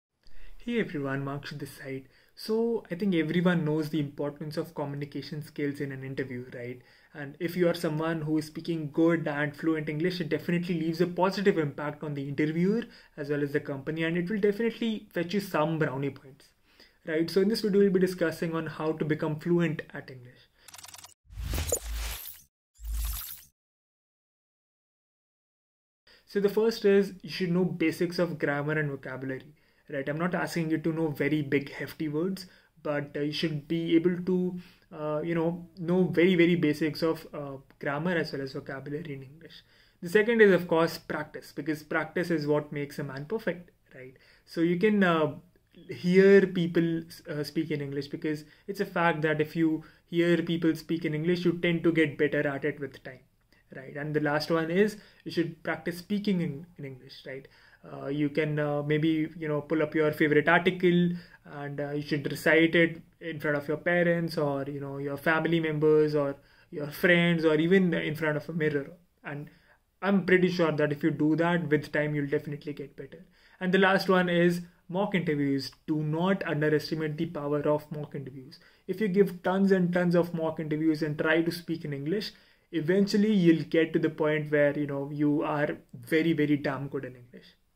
How To Become Fluent in English? | How To Learn English | English Speaking Tips | Coding Ninjas
BEST FREE Resource To Learn DSA, C++, Python, Java, Web Development, Competitive Programming,

This video will talk about How To Become Fluent in English!

How To Become Fluent in English? | Coding Ninjas